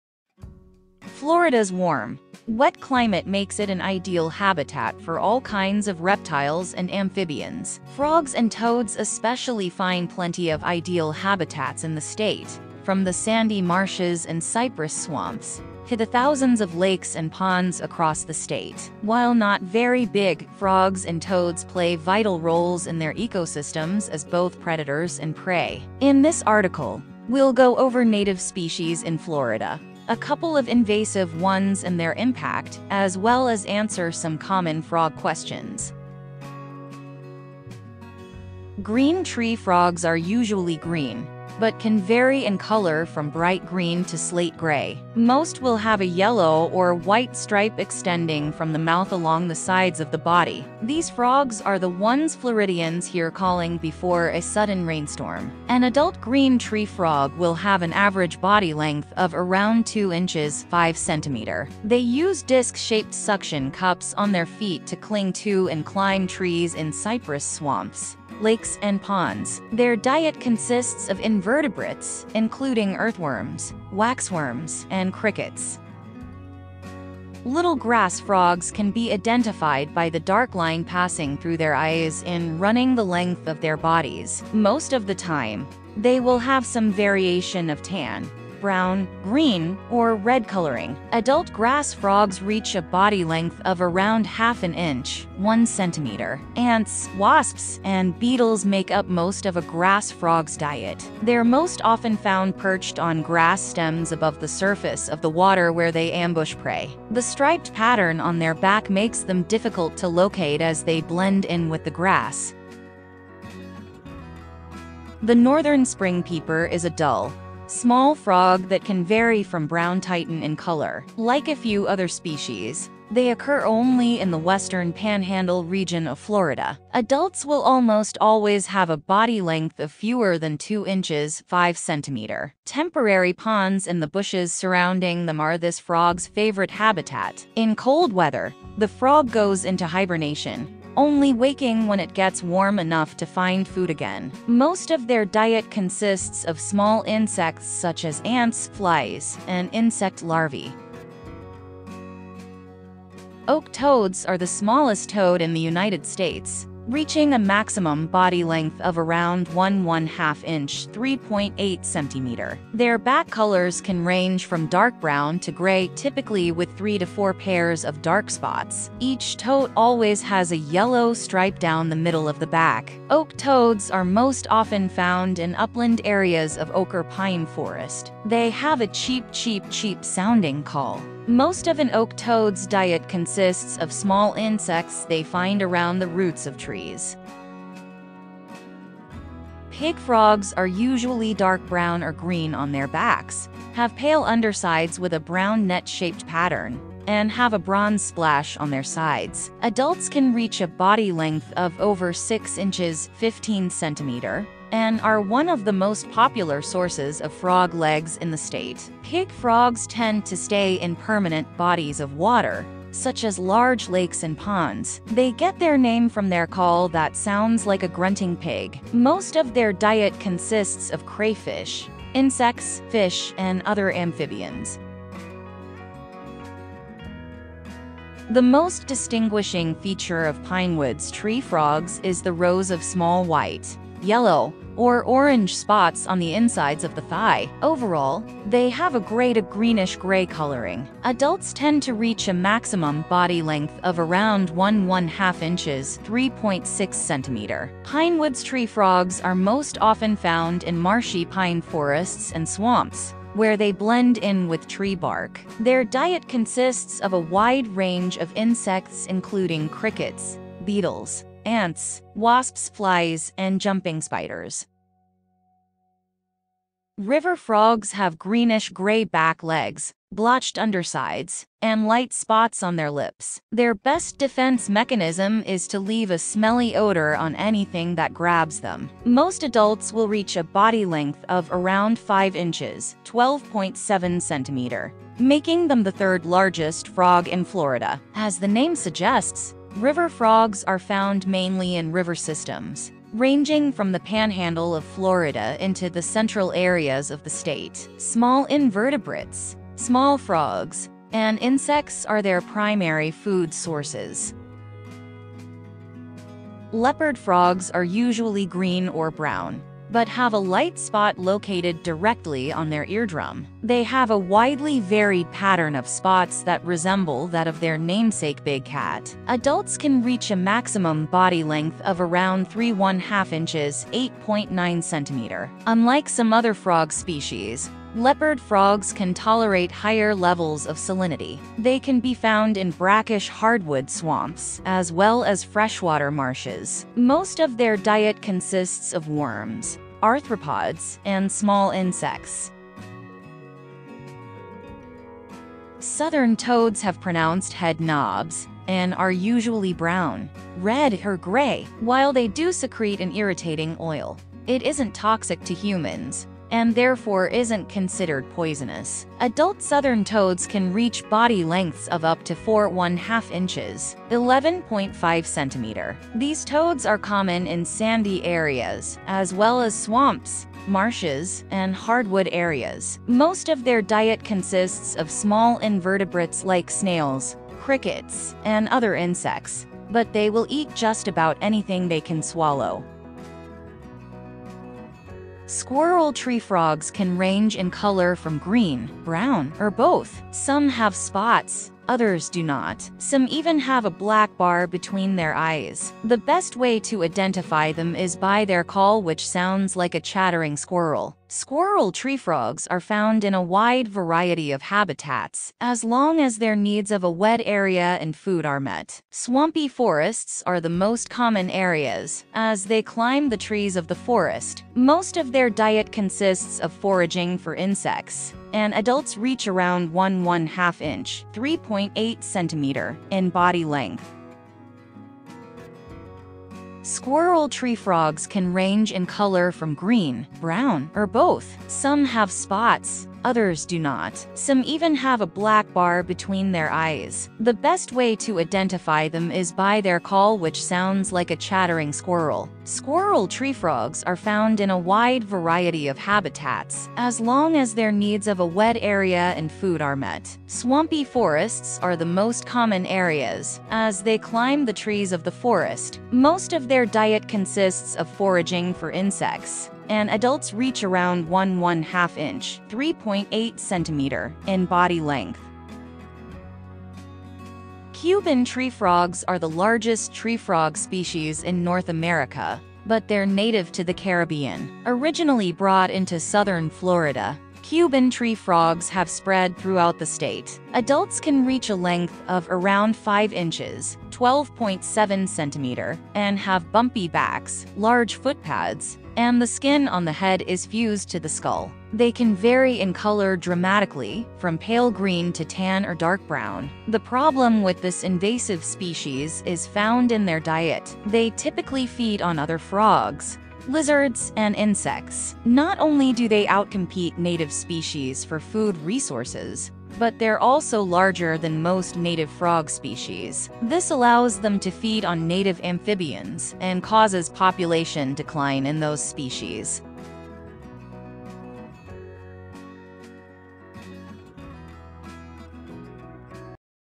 Amazing Florida Frogs And Toads
Frogs and toads are both amphibians, look very similar, and can show up in the same places sometimes. It’s easy to confuse the two, especially since we sometimes use the two words interchangeably in conversation. While similar, frogs and toads are distinctive animals and can be distinguished by some key characteristics.